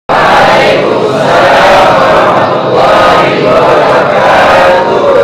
(Kamis, 26.08.21) SUBUH MENGGAPAI KEBERKAHAN | bersama K.H. Luqmanulhakim dan Pak Erie Sudewo
Indonesia Beramal Bukti Bakti untuk Orang Tua

Produced by Munzalan Information Center